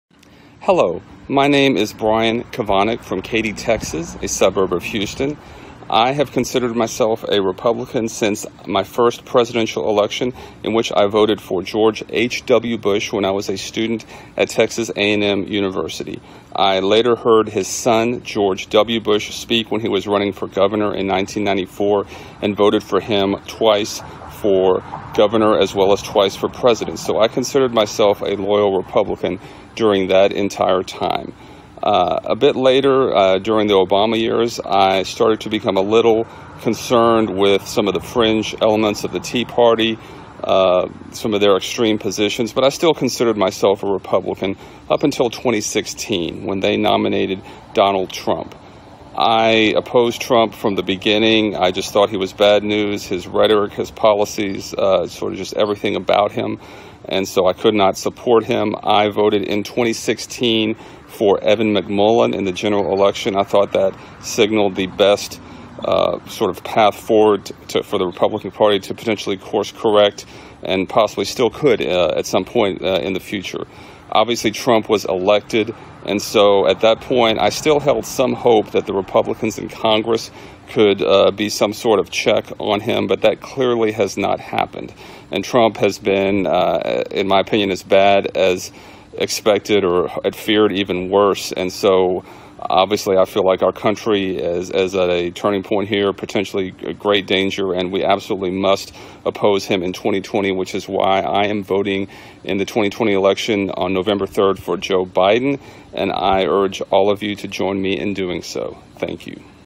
Brian has been a loyal Republican since he was 18-years-old, but Trumpism has left him voting Biden.
"I feel like our country is at a turning point here... which is why I am voting in the 2020 election, on November 3rd, for Joe Biden.